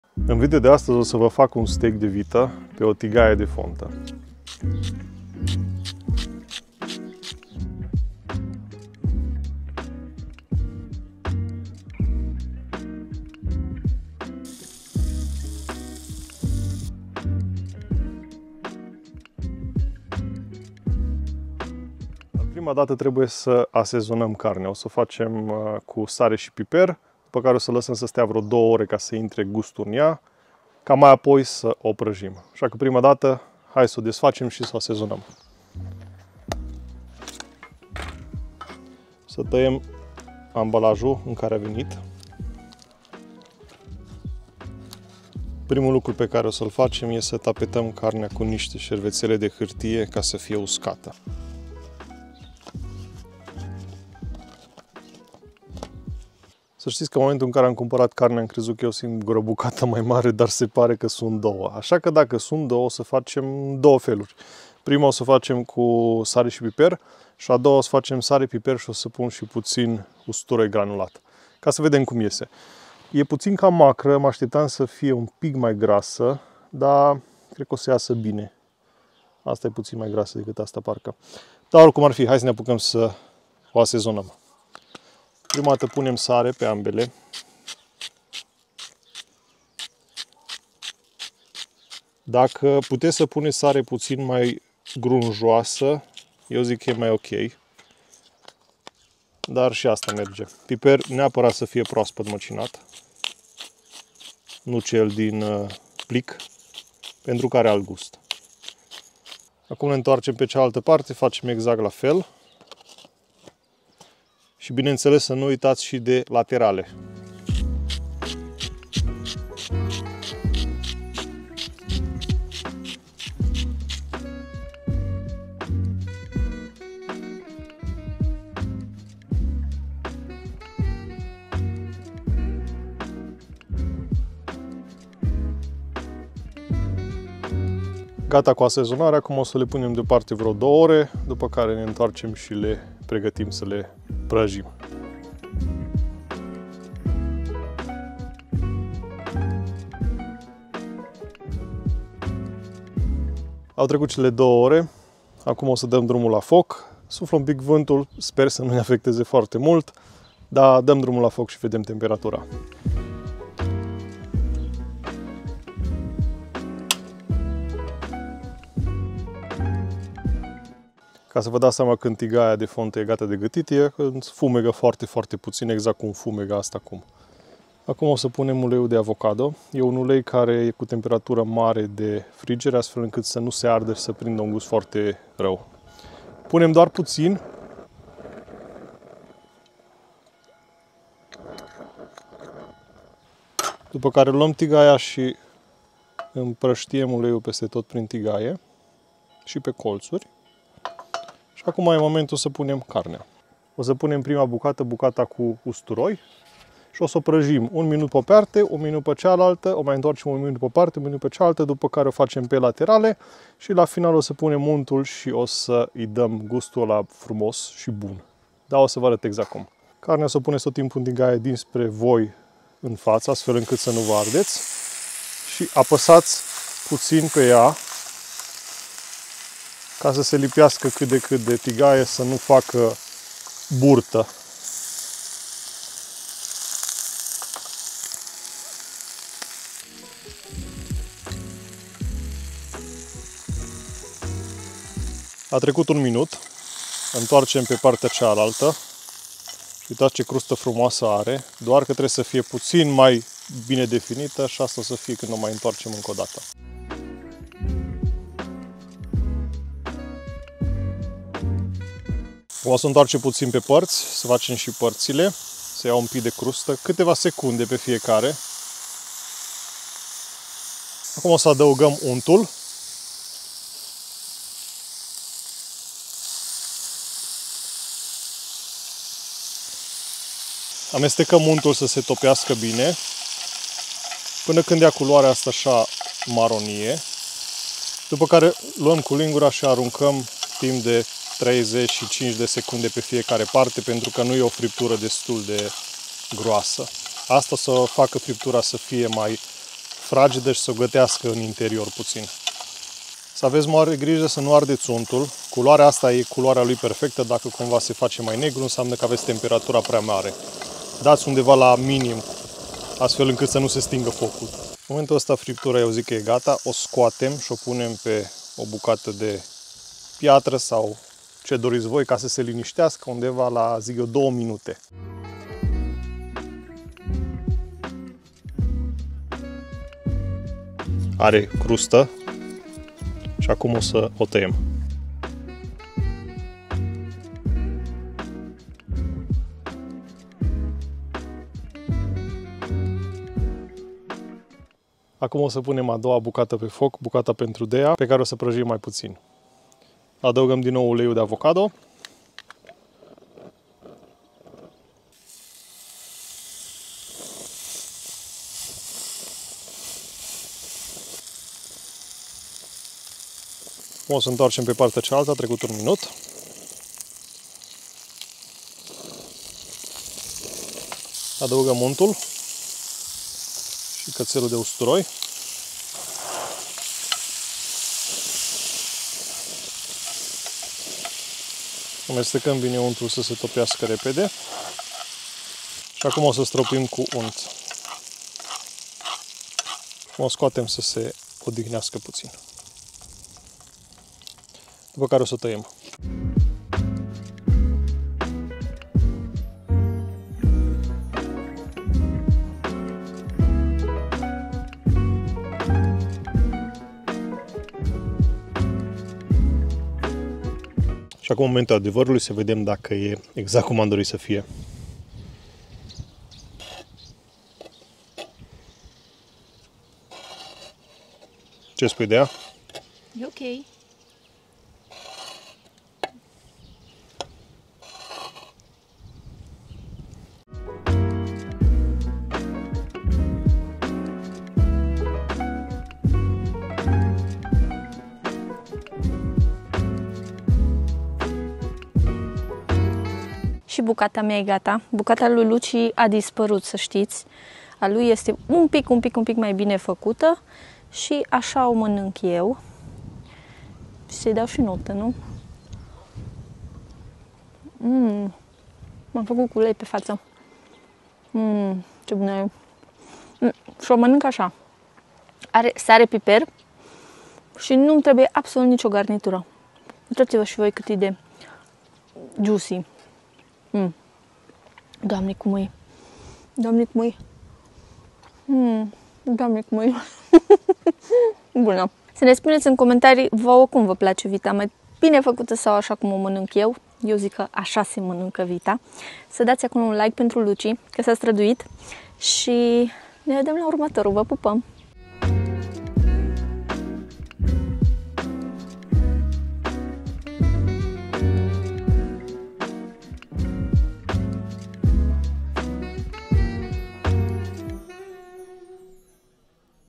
Cum gătești perfect o friptură de vită la tigaie de fontă | STEAK DE VITĂ
Am gătit un steak de vită la tigaie de fontă cu unt și usturoi. Voi cum mâncați vita?